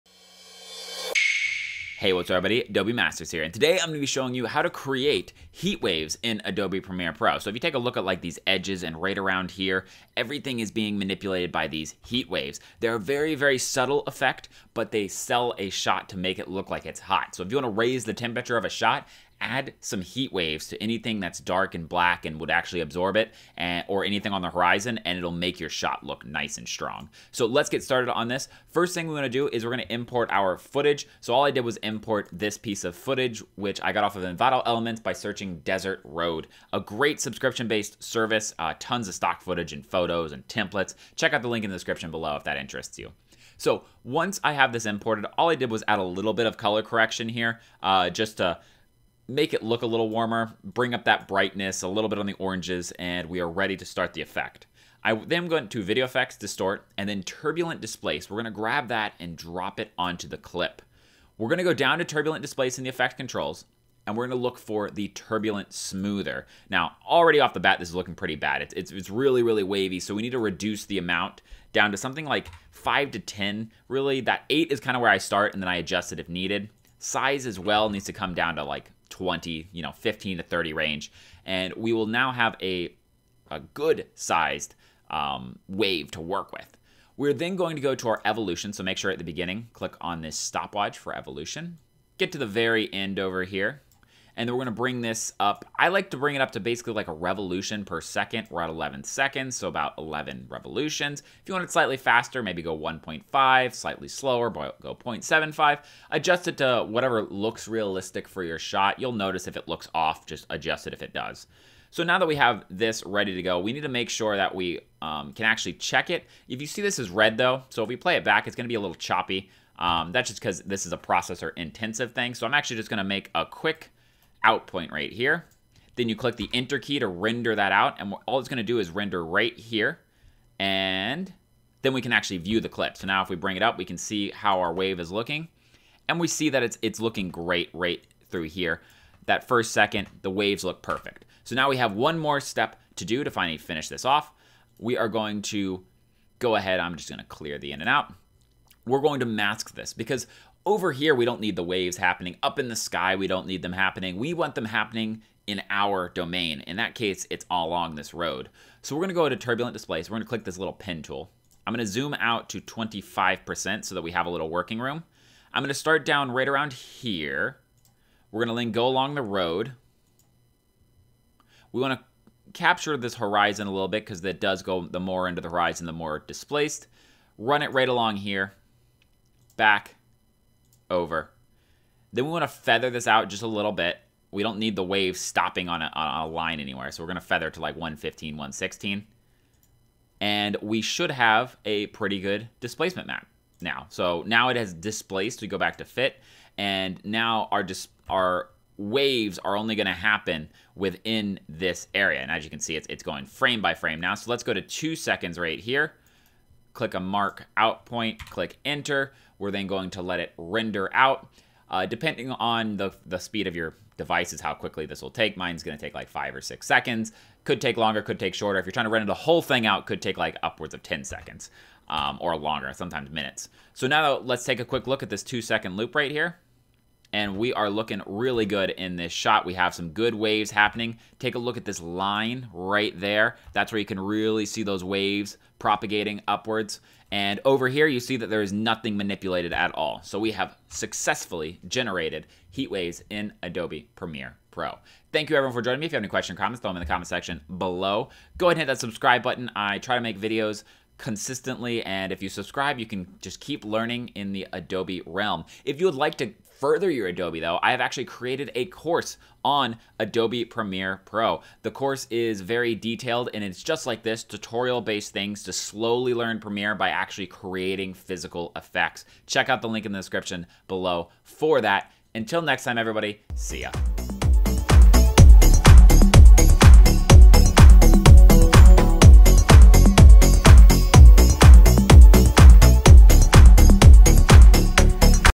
How to Create Heat Waves in Adobe Premiere Pro CC (2022)
Today we go over how to create Heat Waves in Adobe Premiere Pro CC. This is a great skill to have to create a more of a warmer feeling in Adobe Premiere Pro.